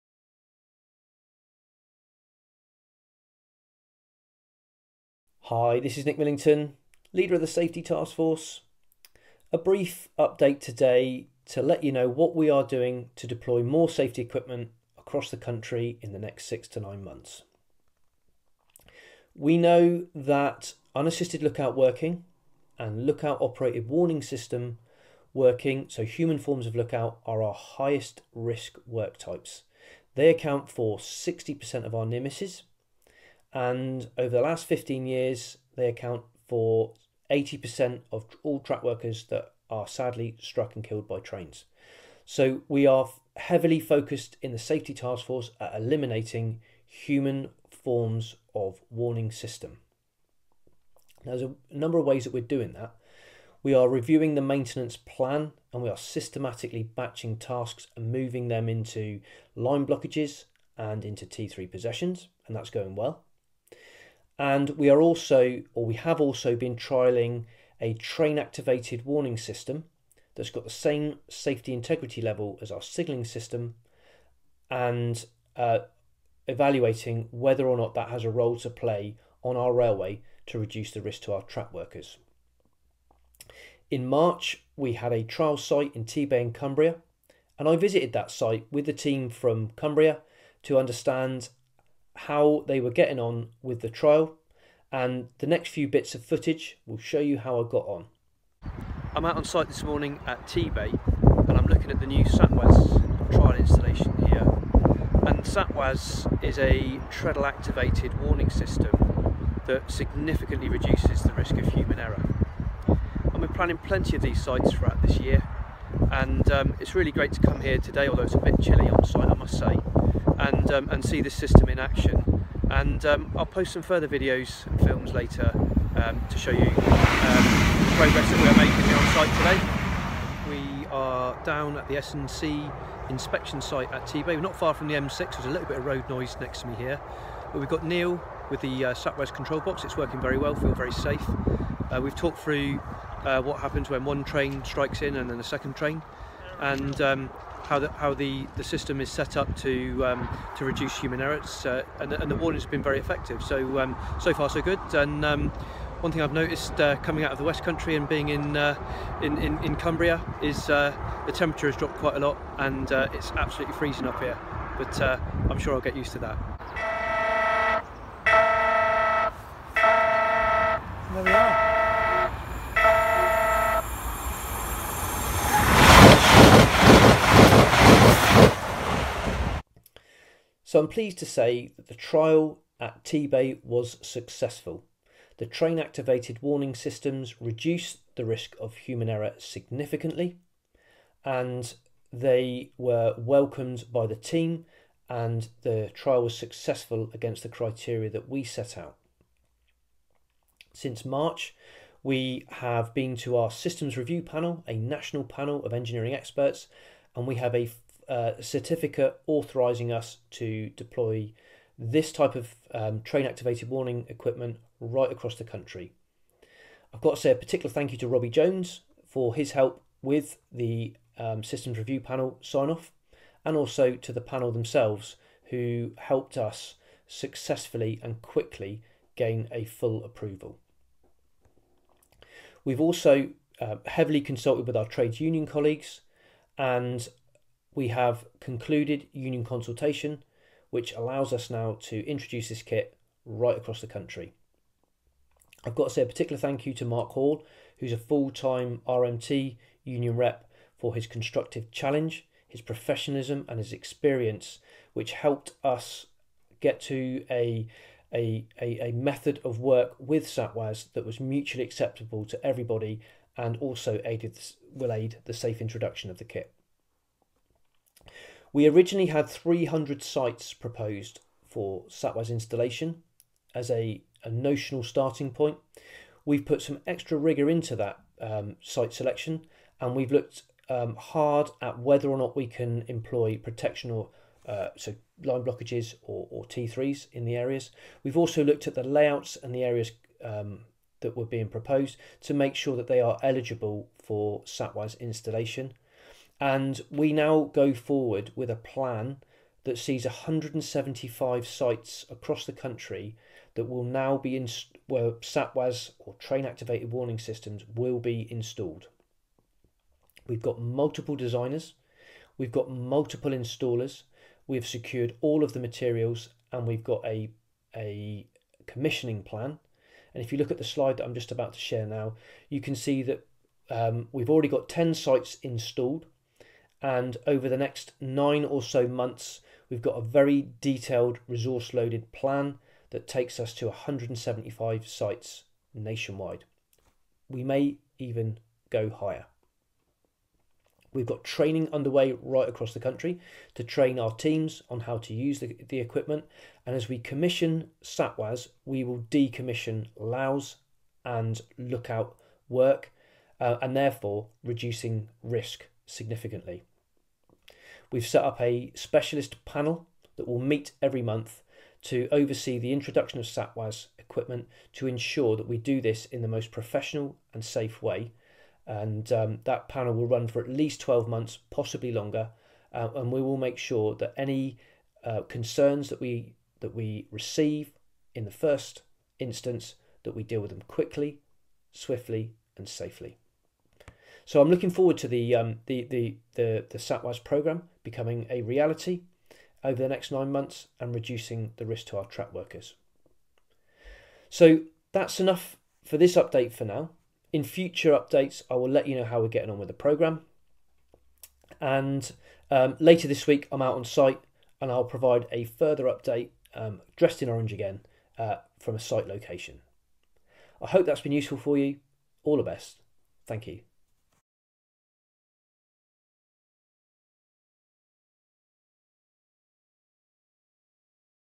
Captain's Vlog Star Date 22 06 21 SATWaS Safety Equipment Update
Another story behind one of my Video Blogs.

Train activated warnings are lower risk than human forms of lookout - But installing train activated systems at scale needed a number of trials to be undertaken. The first trial took place at Tebay and I paid a visit and met the teams who installed and operated the kit. The weather was typically 'Cumbria-ish': Changeable, windy and wet.

In this Video Blog, I met up with people that I hadn't seen for over 12 months due to the various restrictions with COVID - My work in the task force was very safety critical, so I made a number of visits. On occasions, i would be travelling on long trains by myself!

In this Vlog, I was growing in confidence with site filming, editing, bringing a story together with disparate bits of 'B Roll' and passages of film from the Captain's Quarters - Which was my temporary home office i set up in the laundry cupboard at home (and which is still there 4 years later. ).

Deploying technology from conception is hard going in the railway industry and i am grateful to all the team / colleagues in the Safety Task Force who maintained resolve and successfully deployed (and they are still going) an awful lot of kit.

Cheers, Nick